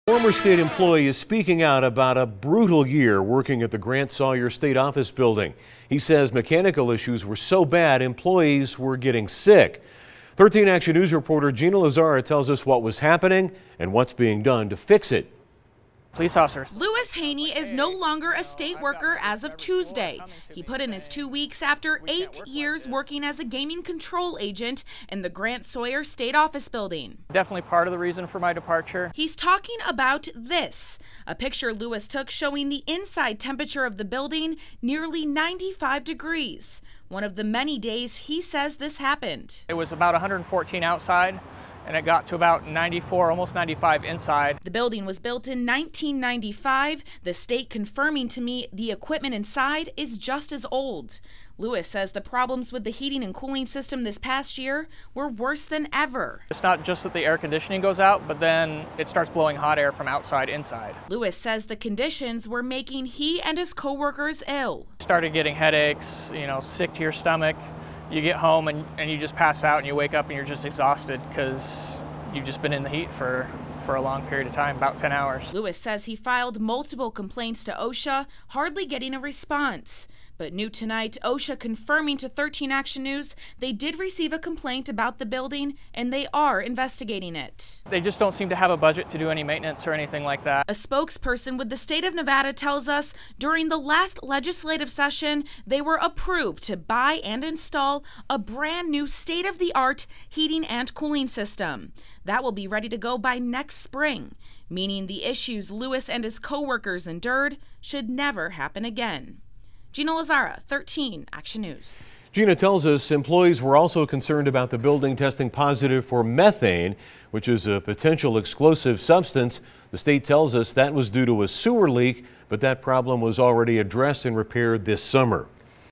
Is Grant Sawyer Building dangerous?
A former state employee says part of the reason he left his job was because of the condition the Grant Sawyer State Office Building is in. Gina Lazara reports.